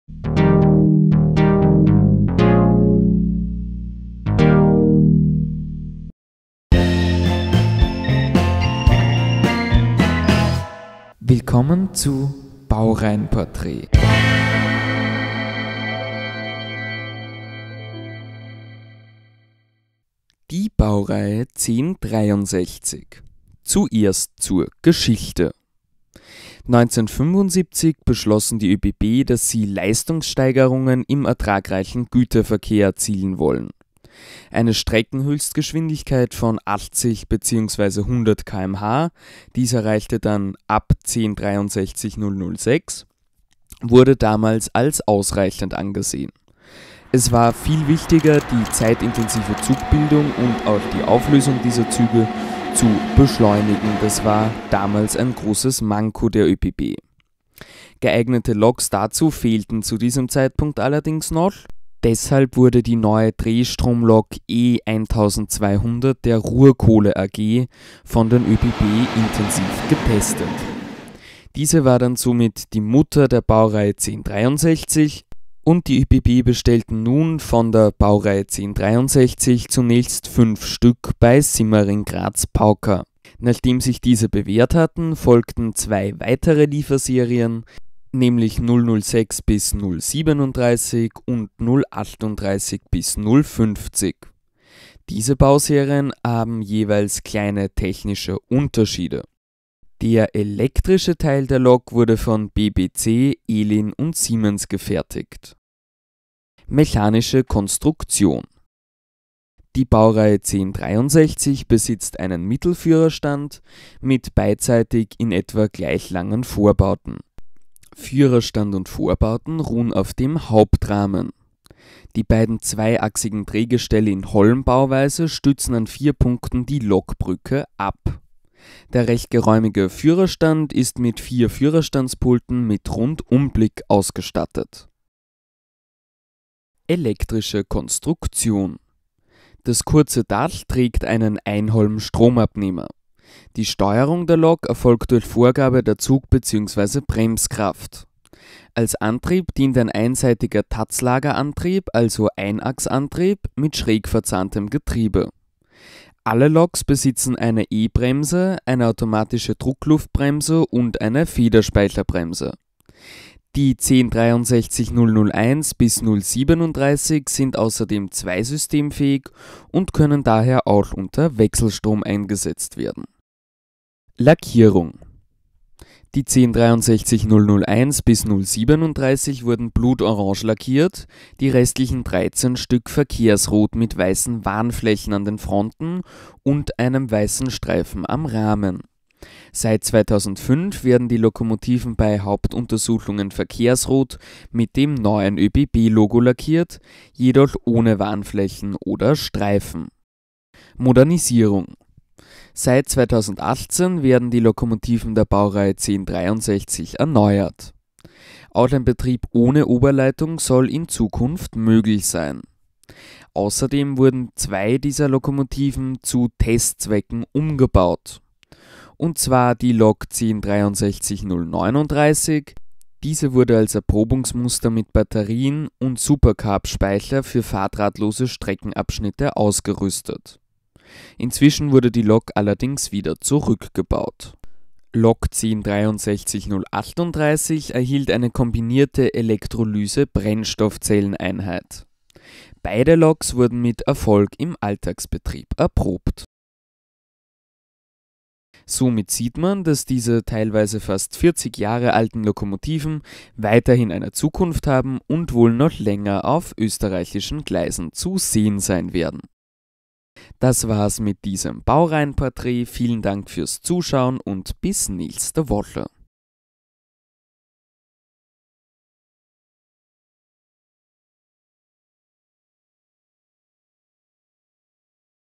Sie werden noch länger fahren! | Baureihenporträt Rh 1063: Geschichte, Konstruktion, Zukunft
Unter der Baureihe 1063 versteht man bei den ÖBB vierachsige Rangierlokomotiven, die großteils schon 35 Jahre alt sind. Umso besser, dass diese schon recht alten Loks ein grundlegendes Refurbishment erhalten, und noch länger bei den ÖBB in Betrieb stehen werden.

Unter der Rubrik Baureihenporträts gibt es in regelmäßigen Abständen Videos zu allen österreichischen Baureihen.